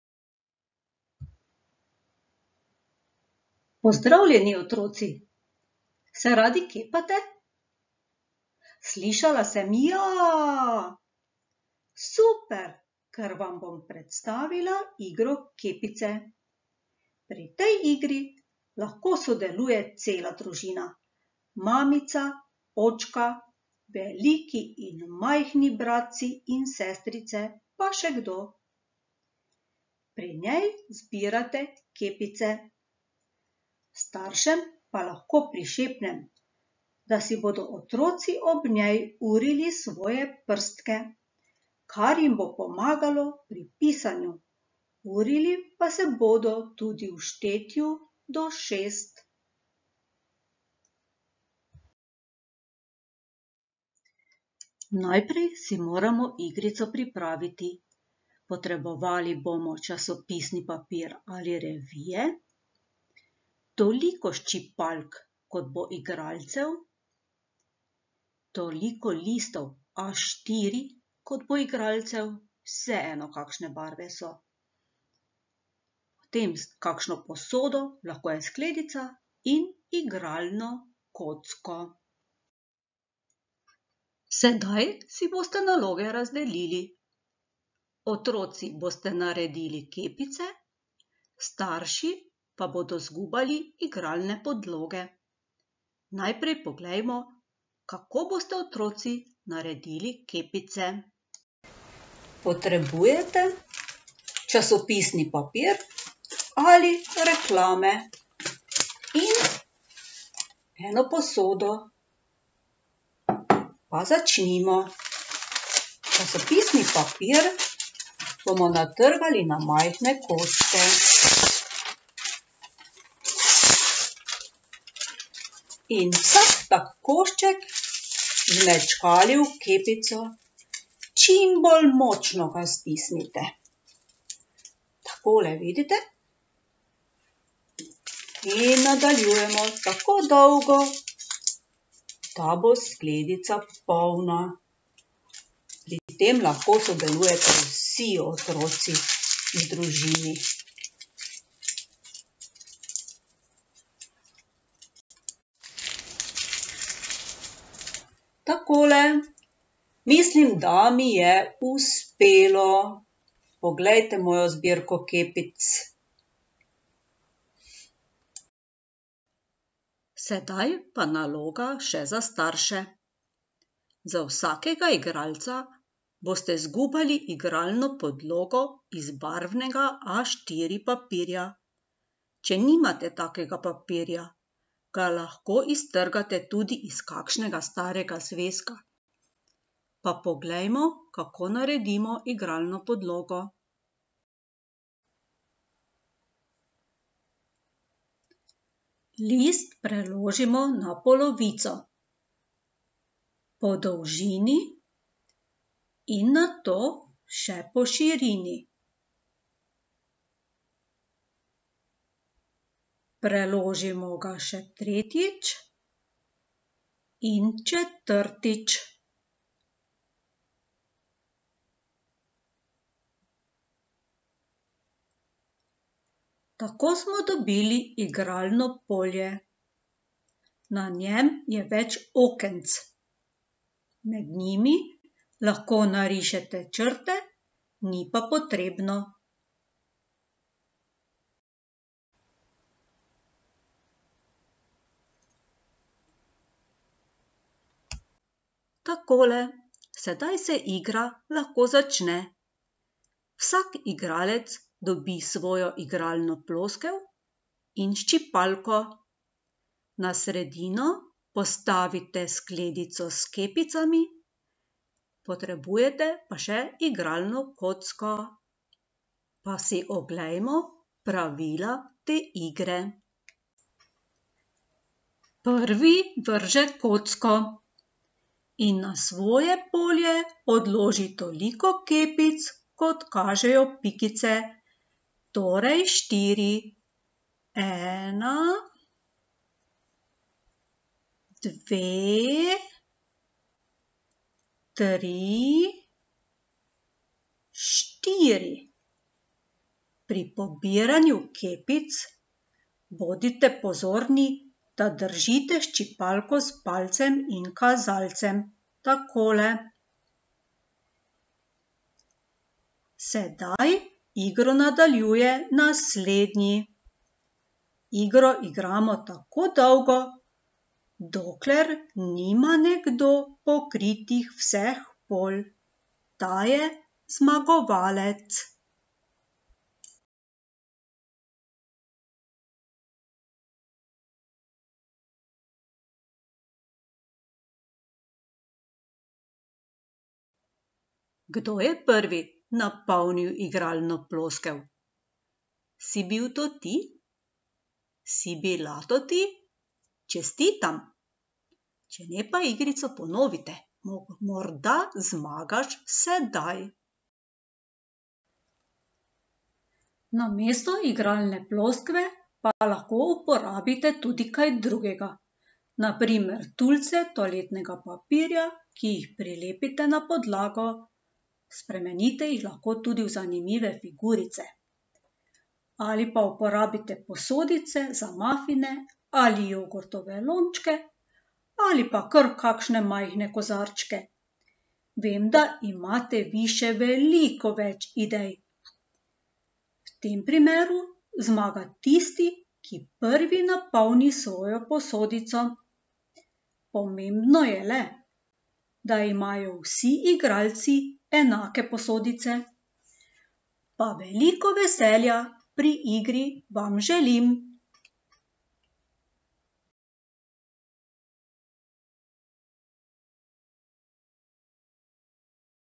Družabna igra "Kepice"
Družabna igra "Kepice" za bodoče učenke in učence 1. razreda.